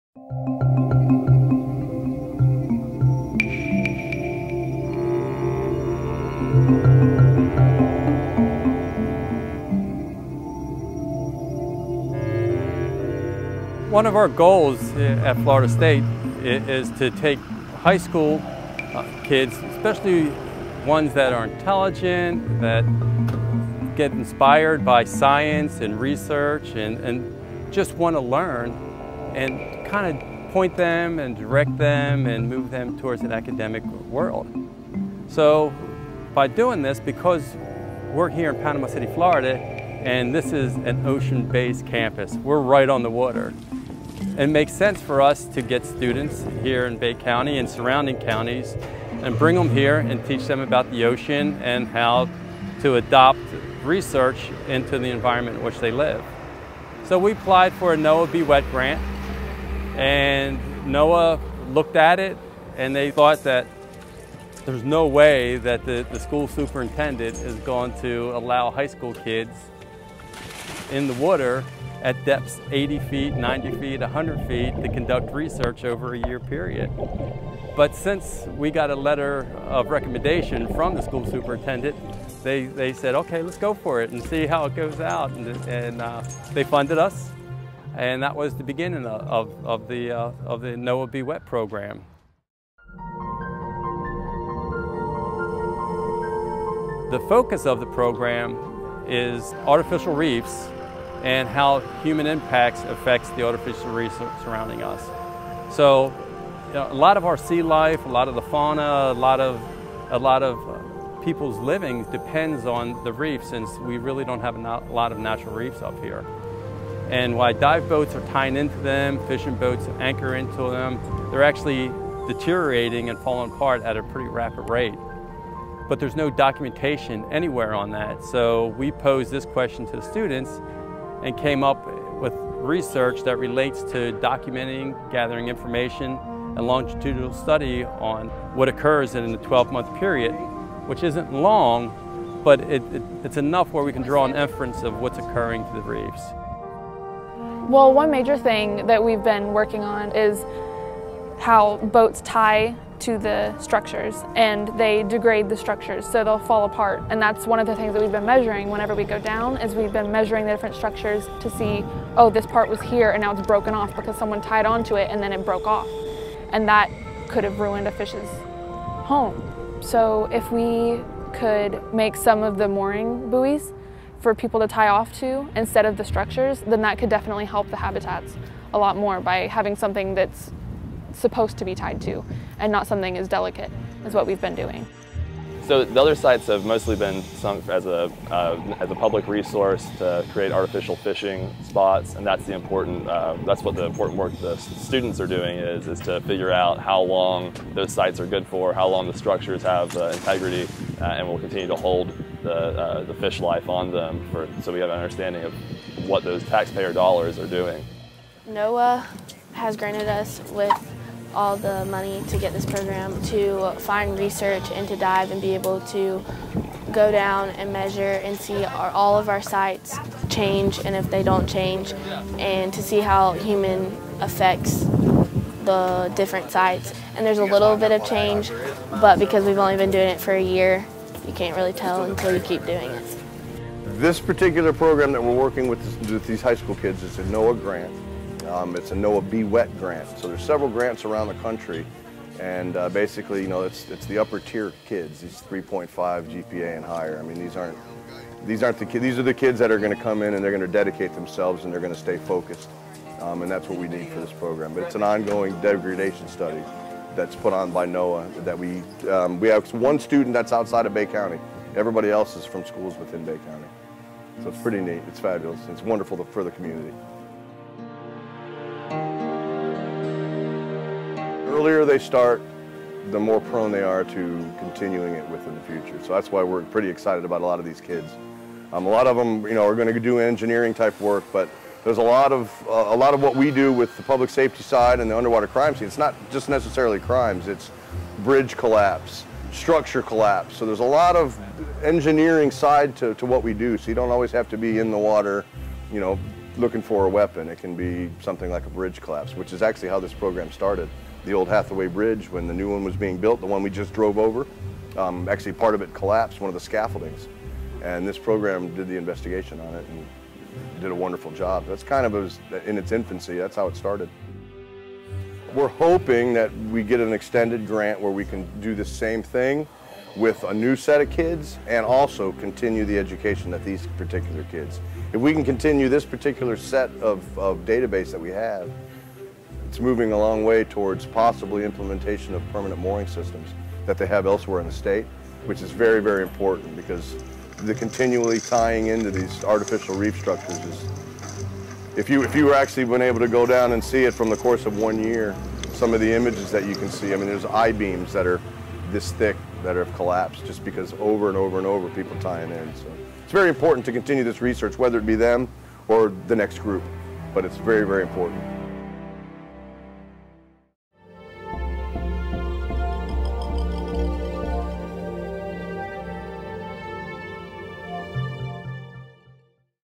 FSU Panama City participates in NOAA B-WET Research
The NOAA B-Wet study is gathering data on artificial reef degradation due to human interaction using traditional underwater scientific methods for fish counts and structural measurements, and incorporated video and SONAR technologies to validate data.  Under guidelines and requirements established by NOAA B-Wet Funded Programs, a group of high school students and teachers are conducting a study of three historical and frequently used dive sites in Panama City Beach, Florida.